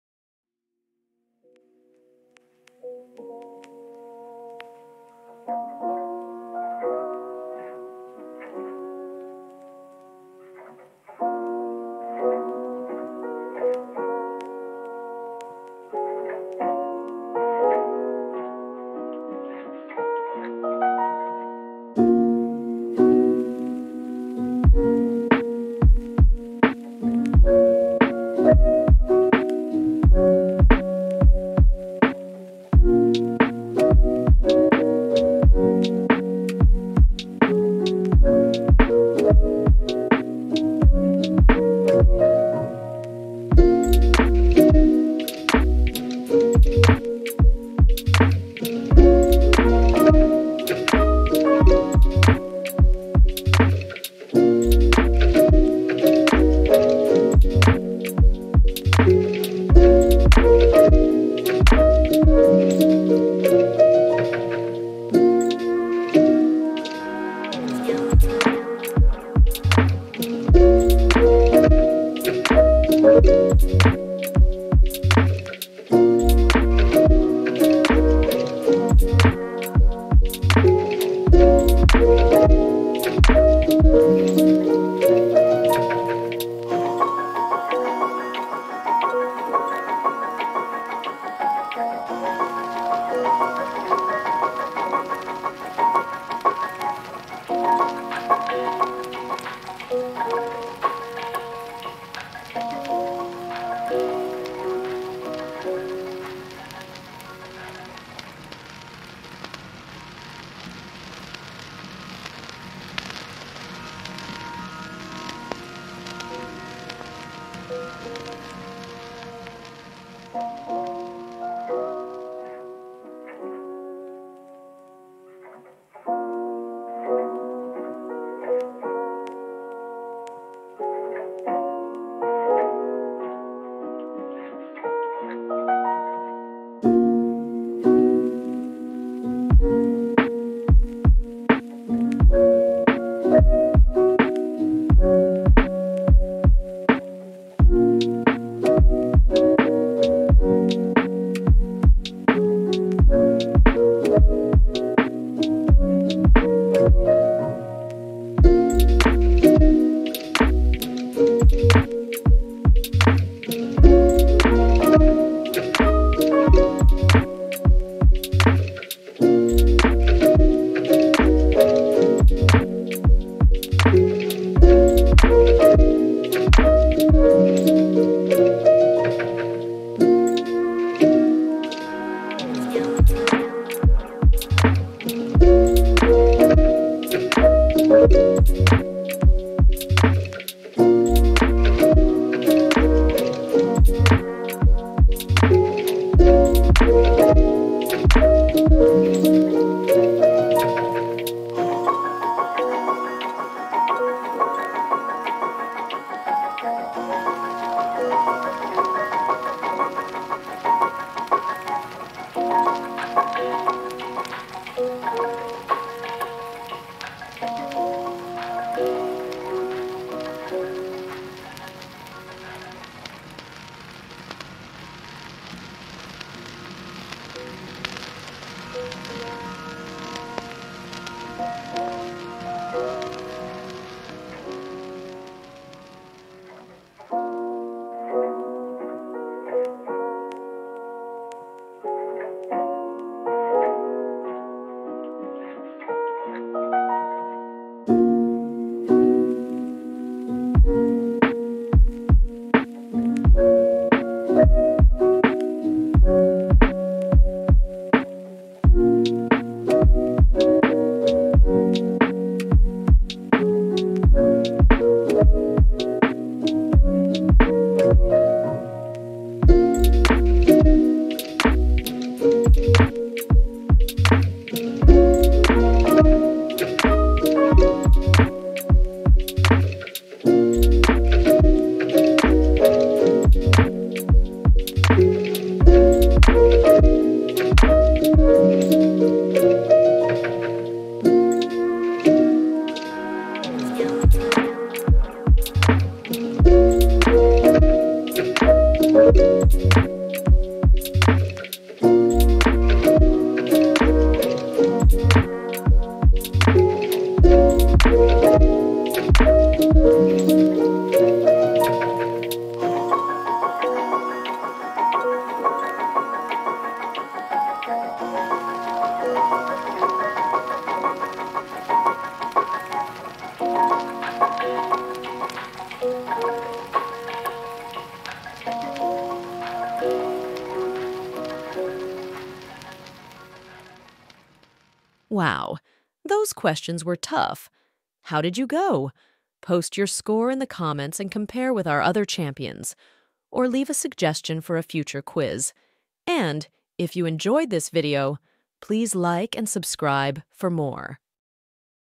Famous Author Quiz: Test Your Literary Knowledge!
Dive into our engaging quiz on famous authors and test your literary knowledge! Explore questions about renowned writers, their works, and their impact on literature. Challenge yourself and discover how well you know the giants of the literary world!